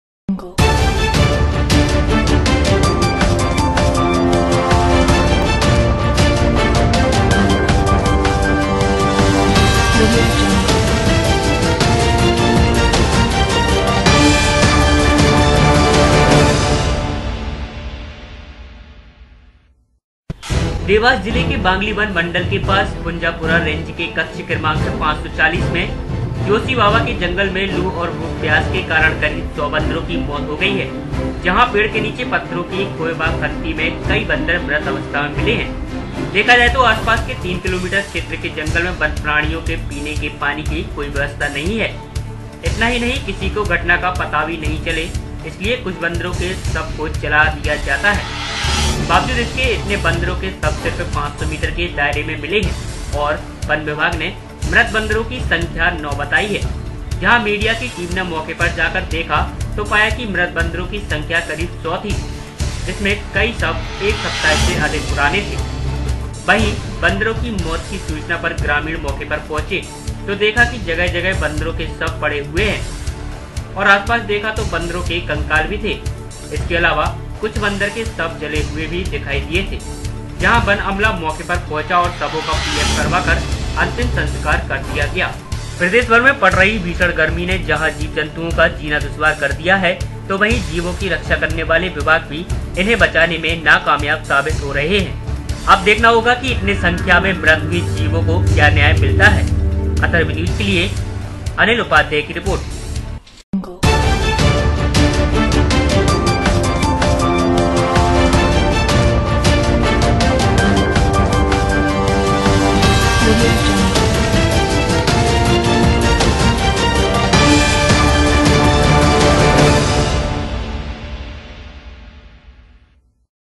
व्यवस्था के अभाव में कई बन्दरों की मौत
व्यवस्थाओ को लेकर होगी जाँच